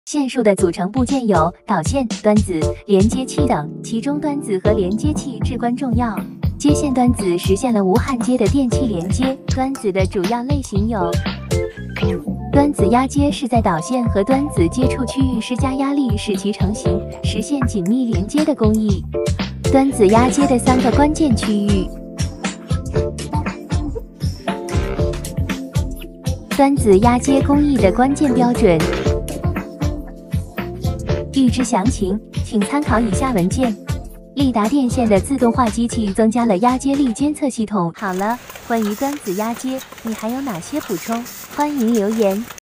Terminal crimping: the decisive link of wire harness quality
In general, the terminal crimping technology level is an important reference to check the manufacturing level of the wiring harness factory. The importance of the terminal is not only to ensure the firmness of the mechanical connection, but also to meet the application requirements of electrical.
Datafield's automation machine to add Crimping force monitoring system that capable to stop machine if detected lower crimpingOr higher crimping force during terminal